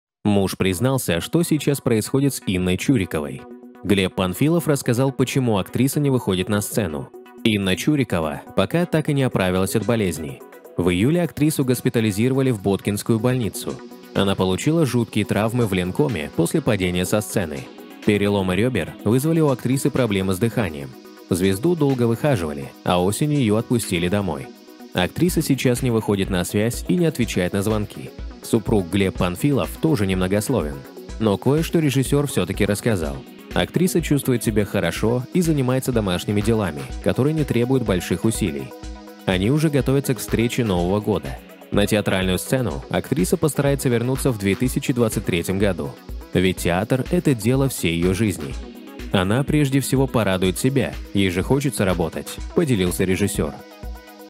Стало известно, почему Инна Чурикова не выходит на связь
Глеб Панфилов рассказал, почему актриса не выходит на сцену. Инна Чурикова пока так и не оправилась от болезни. В июле актрису госпитализировали в Боткинскую больницу. Она получила жуткие травмы в Ленкоме после падения со сцены. Переломы ребер вызвали у актрисы проблемы с дыханием. Звезду долго выхаживали, а осенью ее отпустили домой